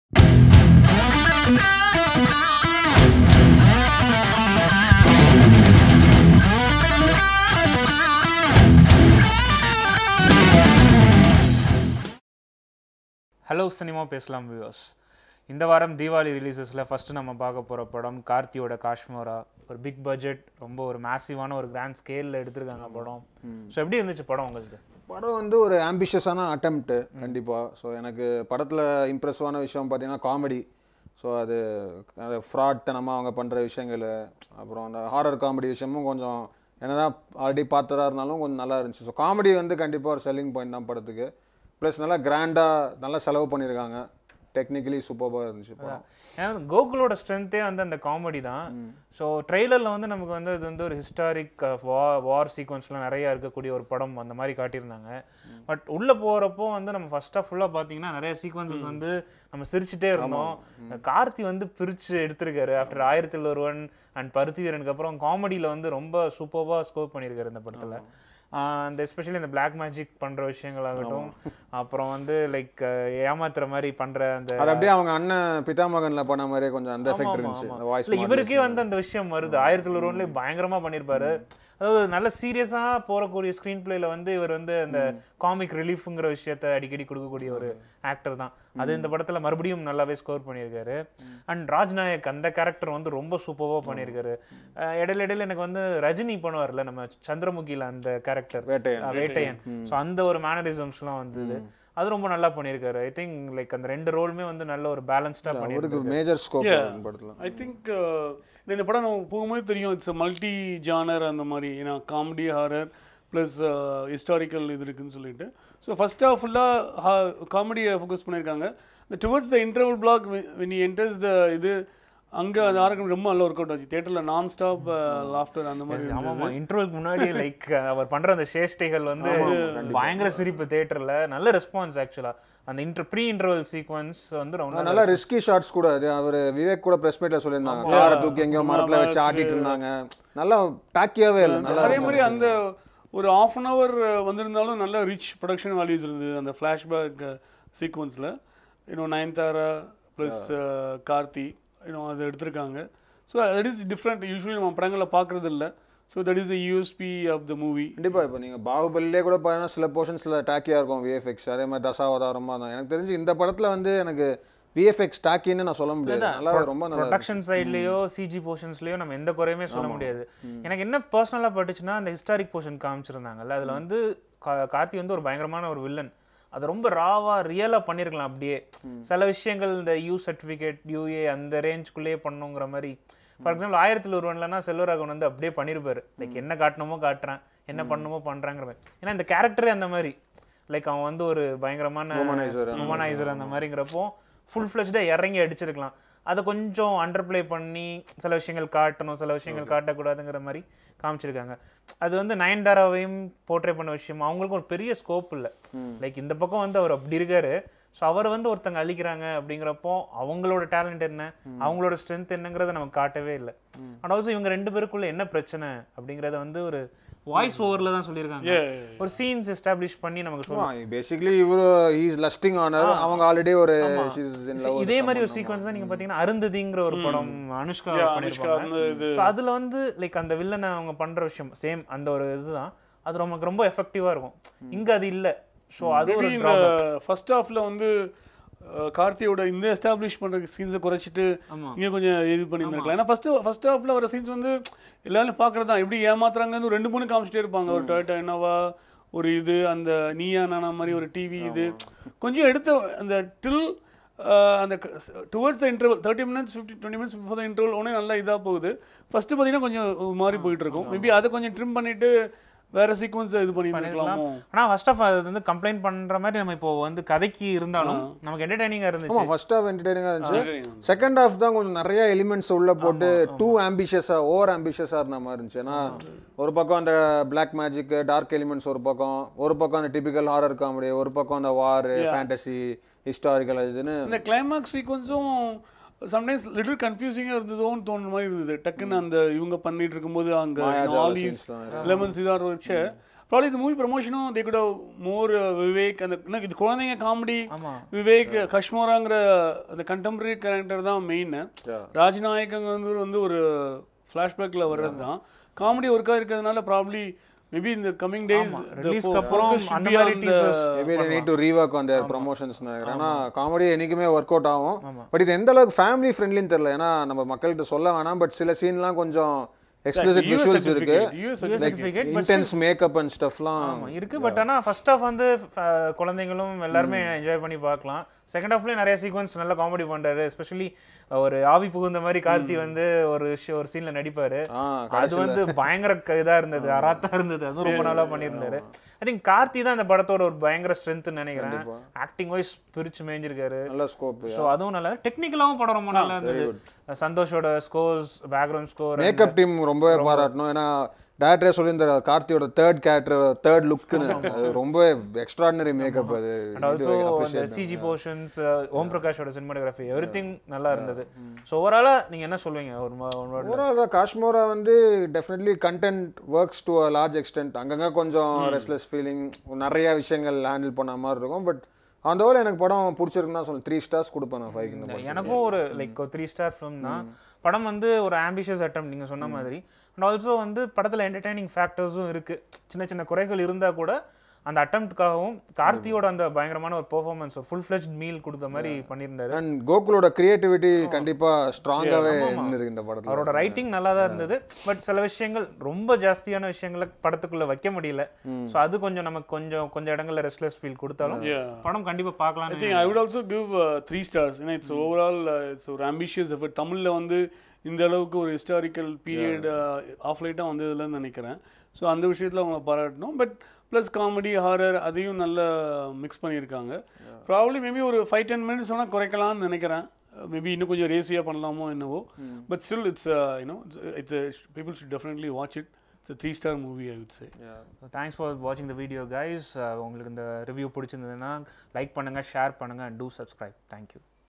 Kaashmora Review by Cinema Pesalam
Cinema Pesalam, Let's Talk Cinema.

A group of passionate movie buffs interact with each other about new releases, trending topics and of course, interesting interviews. In this episode team talk about Kaashmora Movie.

a Divo Presentation